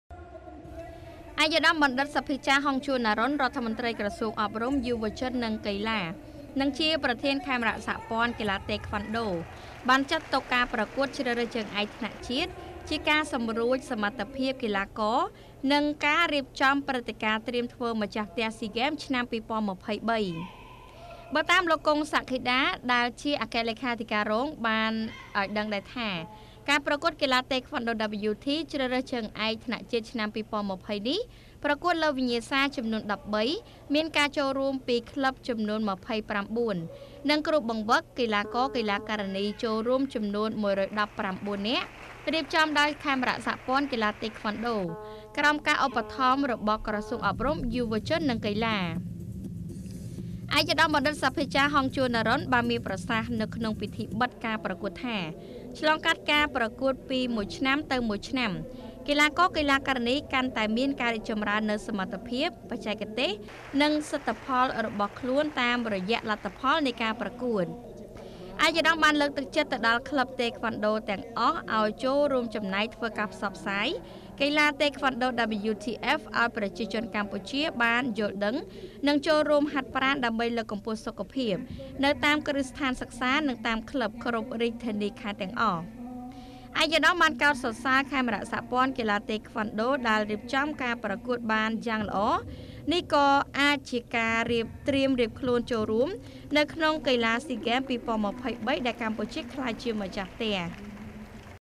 ប្រធាន​សហព័ន្ធ​កីឡា​តេក្វាន់ដូ WTយក​ការ​ប្រកួត​ជើង​ឯក​ថ្នាក់ជាតិ​សម្រួច​ទិស​ដៅ​ទៅស៊ីហ្គេម​ឆ្នាំ​២០២៣
ប្រធាន​សហព័ន្ធ​កីឡា​តេក្វាន់ដូ WT យក​ការ​ប្រកួត​ជើង​ឯក​ថ្នាក់ជាតិ​ សម្រួច​ទិស​ដៅ​ទៅស៊ីហ្គេម​ឆ្នាំ​២០២៣